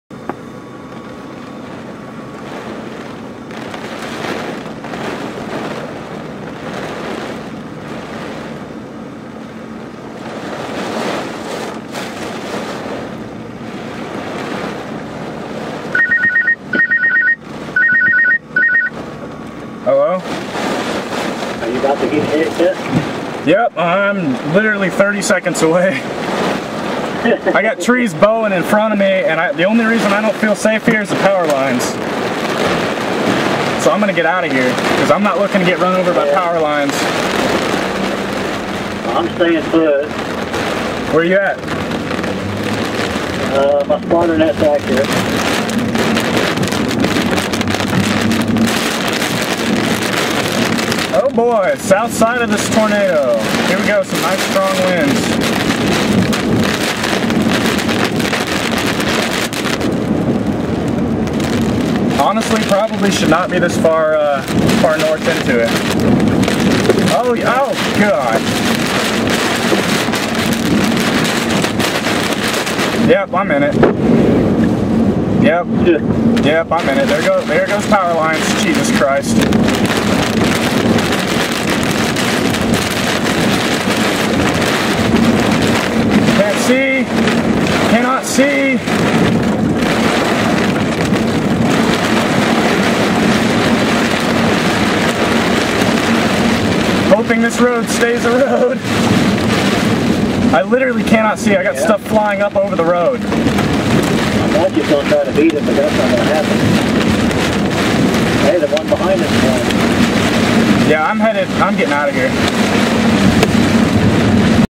Labette, KS Evening Tornado - April 2, 2015
As I was driving east (south of the hook), I noticed a massive lowering to my northwest. I drove out ahead far enough east to jog north and put myself slightly south of the storm's east-southeast motion, to get an idea as to what this lowering was. I ended up in the outskirts of what I believe to be a tornado, as I had multiple dramatic wind shifts after this clip, and powerline flashes behind me as spoken about in the video.

I do not suggest doing what I did in this video, as it could have easily ended up bad if I had not known what I was in for. ALWAYS check your surroundings for things that can become hazardous or block your path, causing you to me immobile and susceptible.